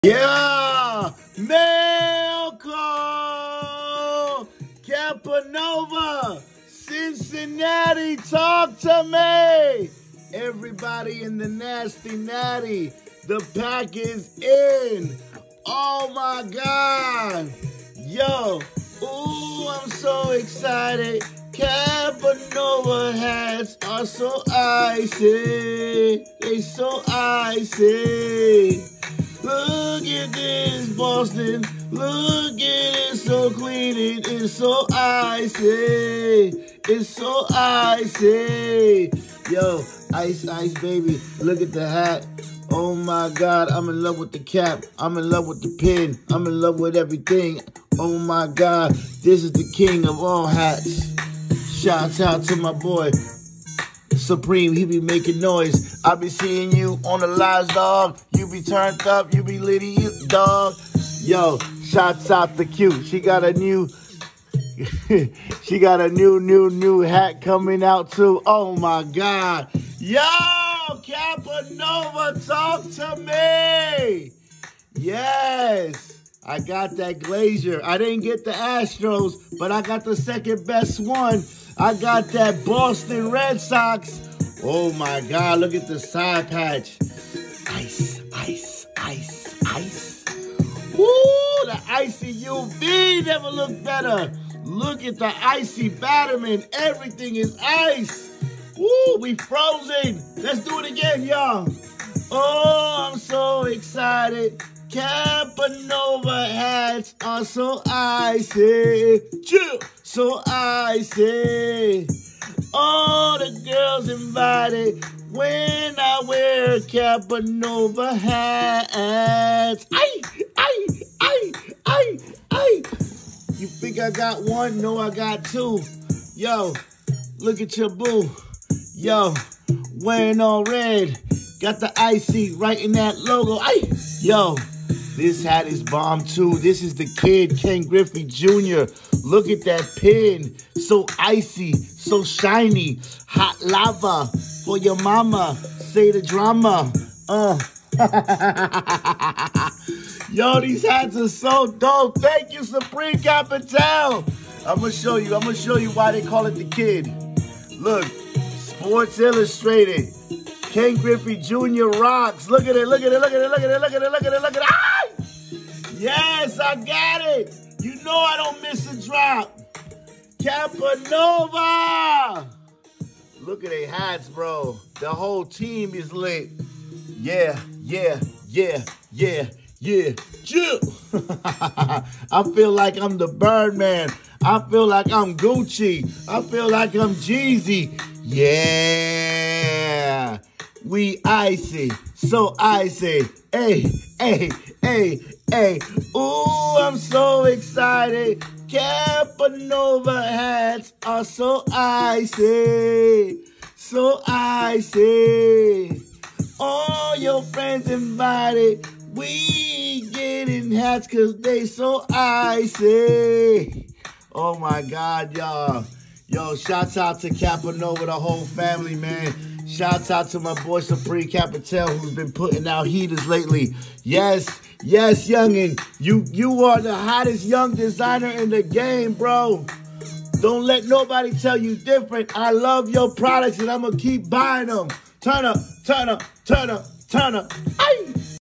CAPANOVA glacier and the kid Mail Call
Capanova hats from Cincinnati are so nice i had to get the Boston Glacier and Reds the kid